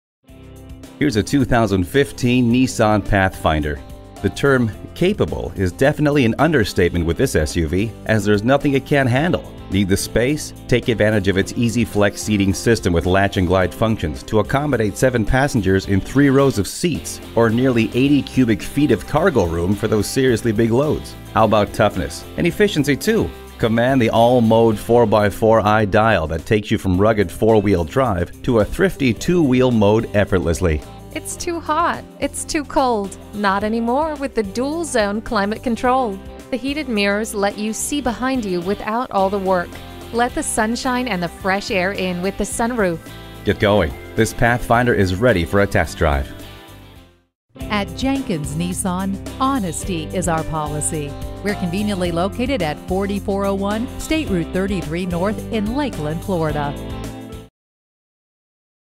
2015 Nissan Pathfinder Lakeland FL Tampa, FL 15P310 - SOLD
Year: 2015
Make: Nissan
Model: Pathfinder
Trim: 2WD 4dr Platinum
Engine: 3.5 L V6 Cyl.
Color: GRAY
Stock #: 15P310
VIN: 5N1AR2MNXFC675644

Address:
401 State Rd 33 N